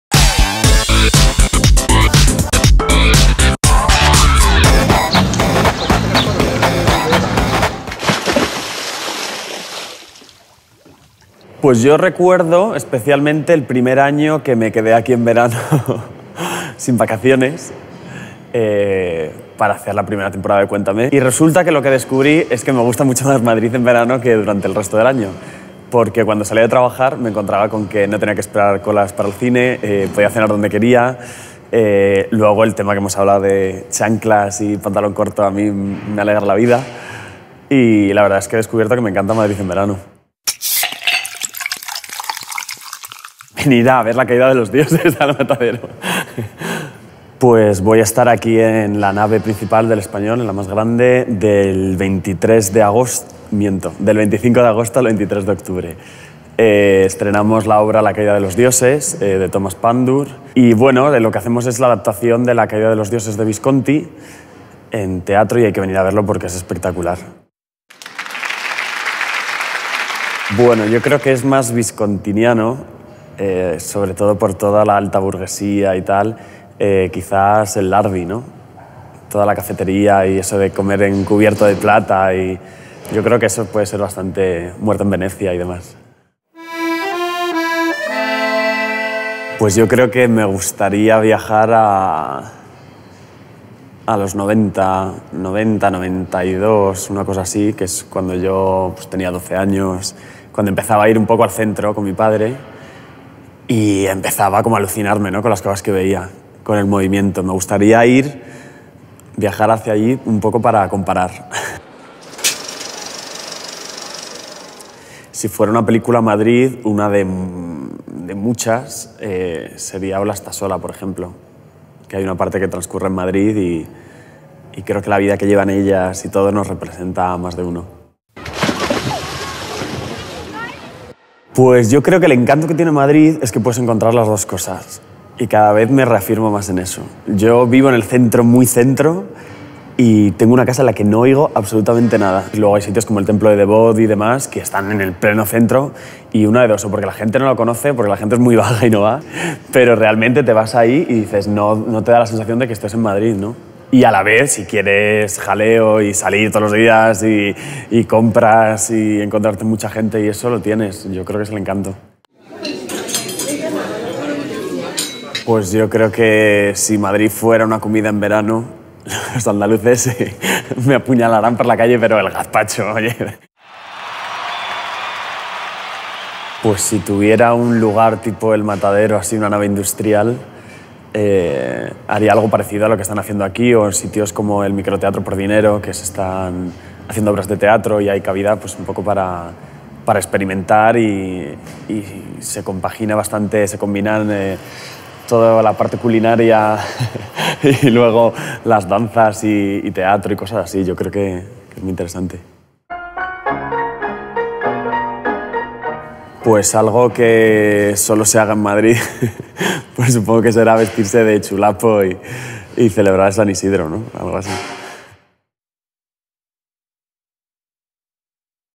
Refréscate con... Pablo Rivero y Matadero Madrid
El joven actor de Cuéntame nos habla desde Matadero Madrid de este espacio donde interpretará este verano La caída de los dioses y de cómo disfrutar del verano en la ciudad.

Beat the heat with... Pablo Rivero and the Matadero Madrid cultural centre

The young actor from the hugely popular TV series Cuéntame chats to us from Matadero Madrid - where this summer he's starring in the play The Damned- and gives us tips for making the most of the hot season.